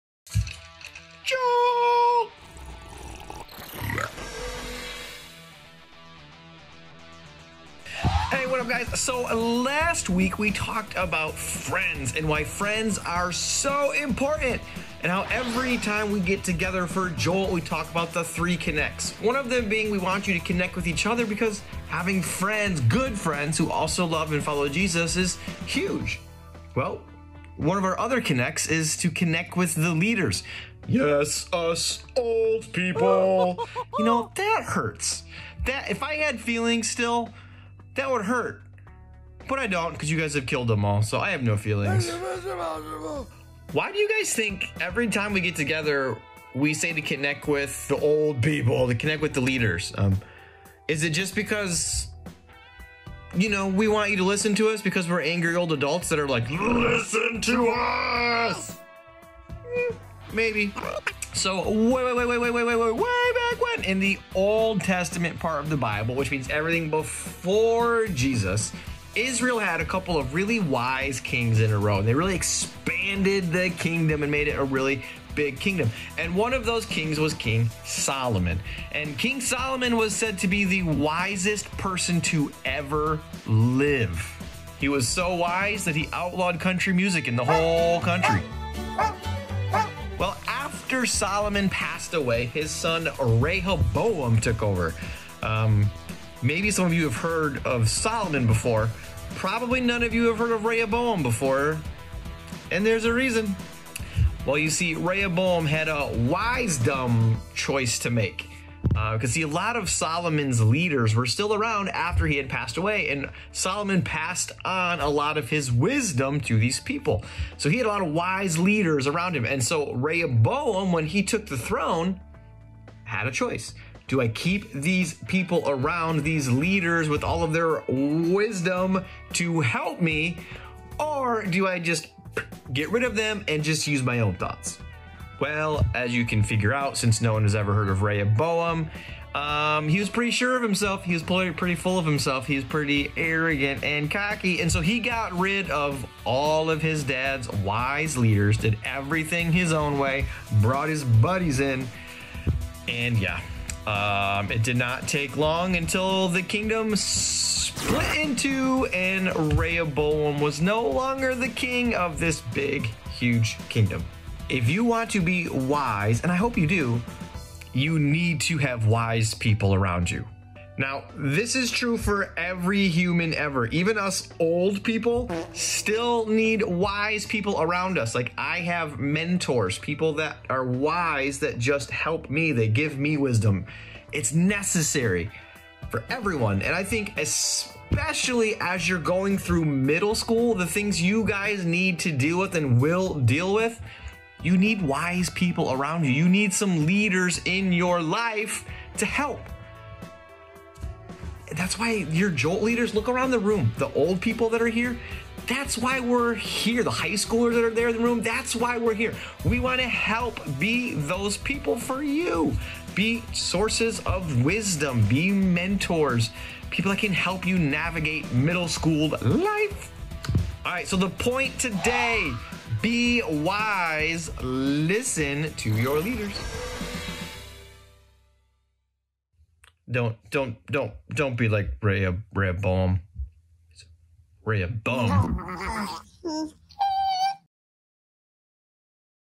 Jolt | Wize-Dum | Message Video, Week One | July 07, 2024
Hey parents and students of Jolt, check out this first video as our friend Brandon from JoltTV, introduces our new series for the month of July called Wize-Dum. Let's watch and see what he has for us today.